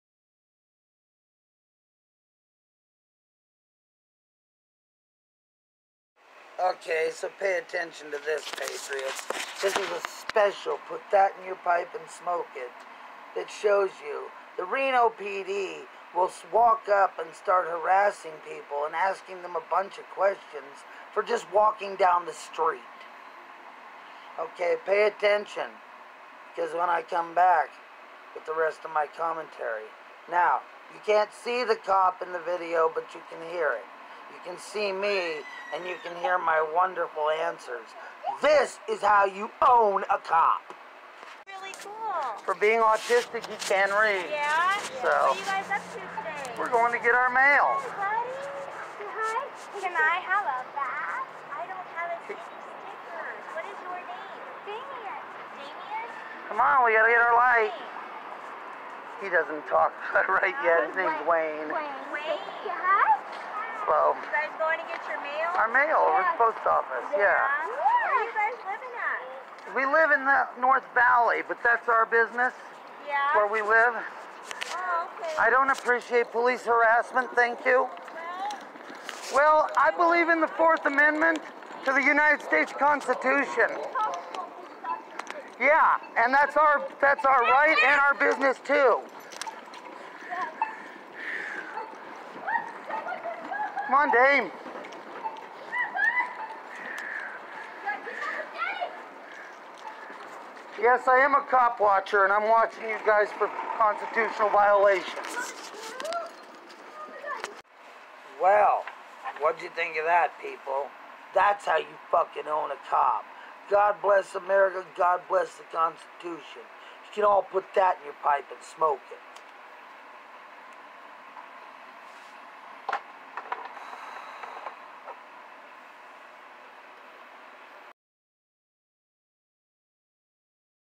Reno cop gets owned see description🇺🇸🇺🇸🇺🇸🇺🇸🇺🇸🇺🇸🇺🇸🇺🇸🇺🇸🇺🇸🇺🇸🇺🇸🇺🇸🇺🇸🇺🇸🇺🇸
I was walking down the street to go to the post office with my family. Went out of nowhere this cop walks up behind me and starts asking us questions. My response is classic. Reno PD can suck my American constitutional dick. 🇺🇸🇺🇸🇺🇸🇺🇸🇺🇸🇺🇸🇺🇸🇺🇸🇺🇸🇺🇸🇺🇸🇺🇸🇺🇸🇺🇸🇺🇸